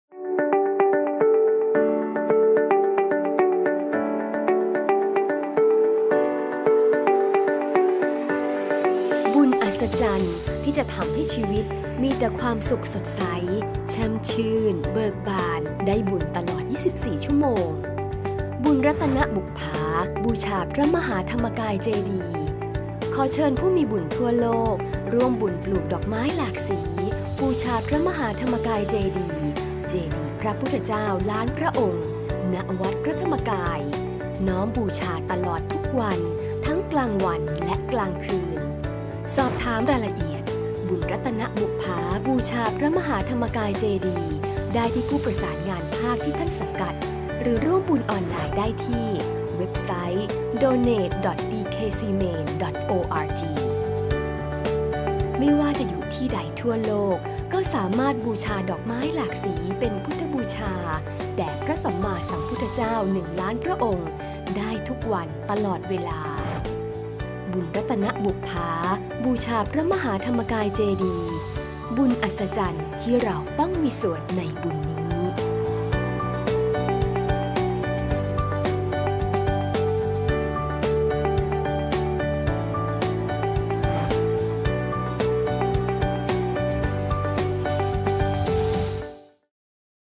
New!!! spot “บุญรัตนบุปผา บูชาพระมหาธรรมกายเจดีย์”
“บุญรัตนบุปผาบูชาพระมหาธรรมกายเจดีย์”
บุญอัศจรรย์ ที่จะทำให้ชีวิตมีแต่ความสุข สดใส แช่มชื่น เบิกบาน ได้บุญตลอด  24 น.

ขอเชิญผู้มีบุญทั่วโลก ร่วมบุญปลูกดอกไม้หลากสี บูชาพระมหาธรรมกายเจดีย์ เจดีย์พระพุทธเจ้าล้านพระองค์ ณ วัดพระธรรมกาย น้อมบูชาตลอดทุกวัน ทั้งกลางวันและกลางคืน

สอบถามรายละเอียด “บุญรัตนบุปผาบูชาพระมหาธรรมกายเจดีย์” ได้ที่ผู้ประสานงานทุกภาคที่ท่านสังกัด หรือร่วมบุญออนไลน์ ได้ที่

ไม่ว่าจะอยู่ที่ใดทั่วโลก ก็สามารถบูชาดอกไม้หลากสี เป็นพุทธบูชา แด่พระสัมมาสัมพุทธเจ้า 1 ล้านพระองค์ได้ทุกวันตลอดเวลา

บุญอัศจรรย์ที่เราต้องมีส่วนในบุญนี้